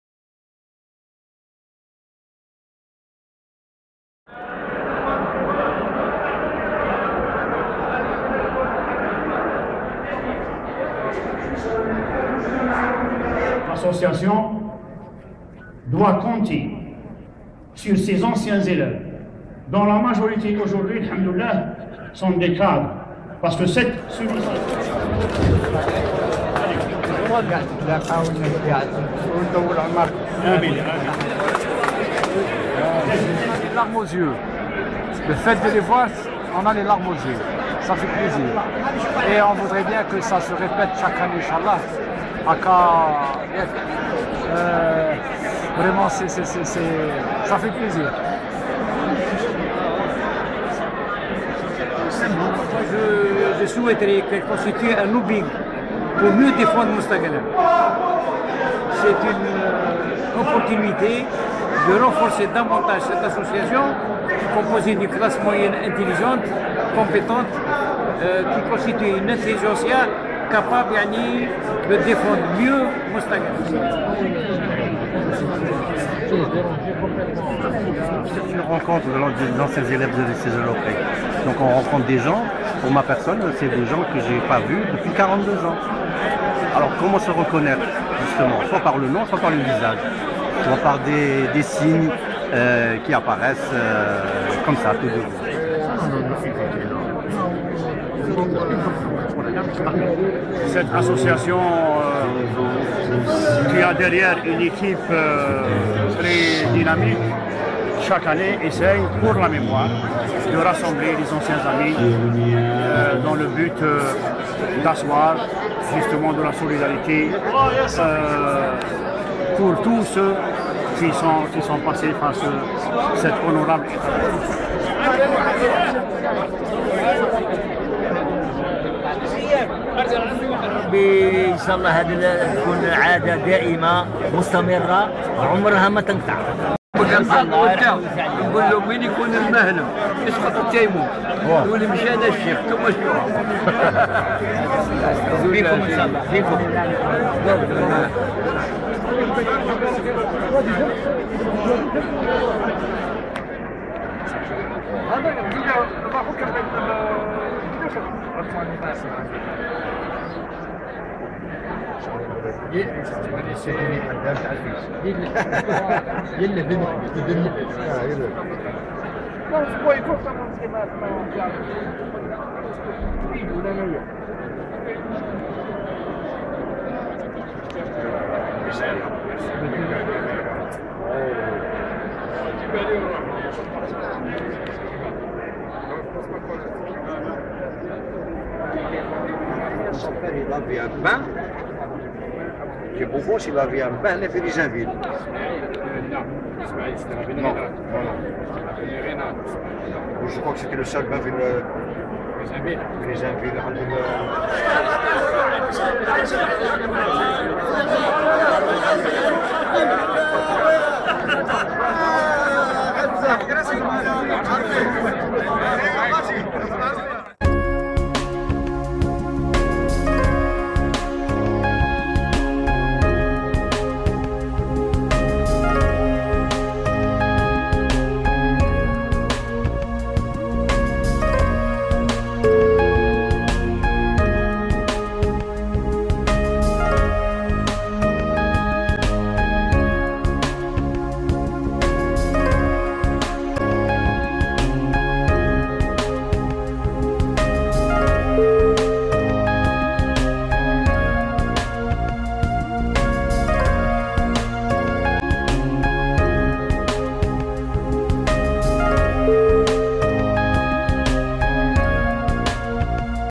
Anciens de Zerrouki - 2014
Le Lycée Zerrouki Cheikh Ibn Eddine, ex-René Basset, a réuni en 2014 les anciens élèves de cet établissement phare de la ville, qui a fait longtemps la fierté de la wilaya et de la région Ouest du pays.